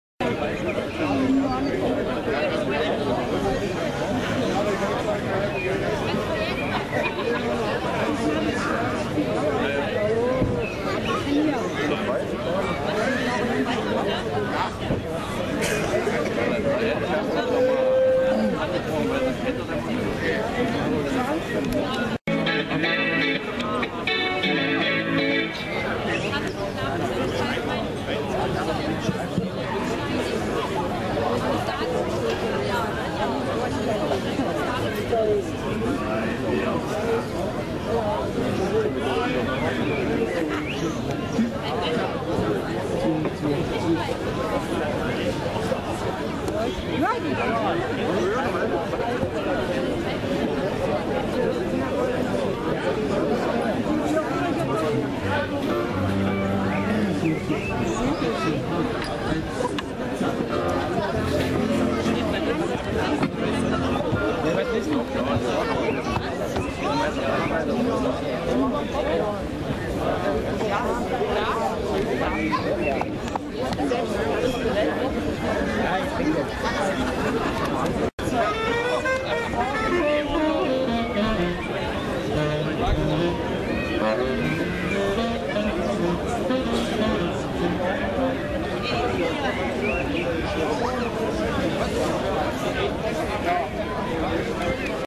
The Firebirds
The Firebirds beim Peter und Paul Fest in Delitzsch